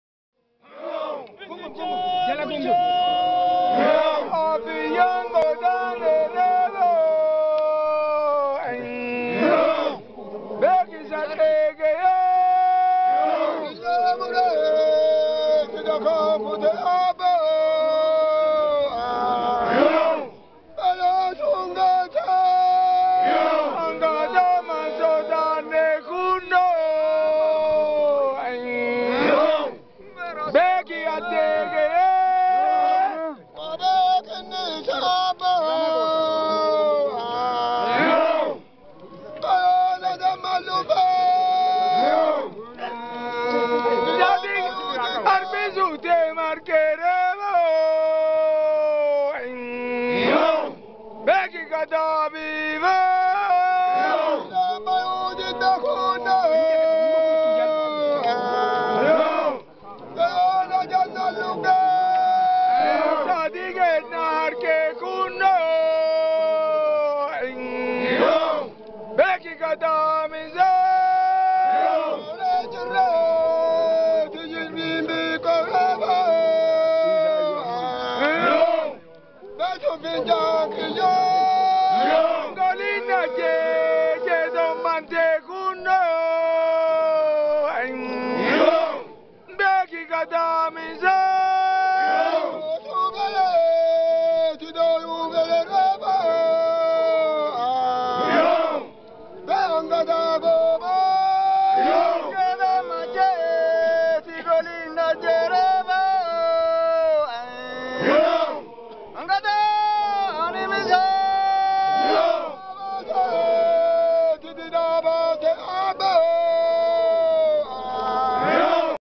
OROMOO GUJII , OLYAAD BARIISOOTIIN
GIKKISNI MALLATTOO GOOTUMMAATI, DHAADATANII BAHU INJIFATANII GEERARU;OROMOONNI,DIINA SABNI HUBANNOODHAN FARRAJEFF, MEE GIKKISA KANA DAAWWADHAA, GUJII OROMOO, ME'EE BOKKOOTTI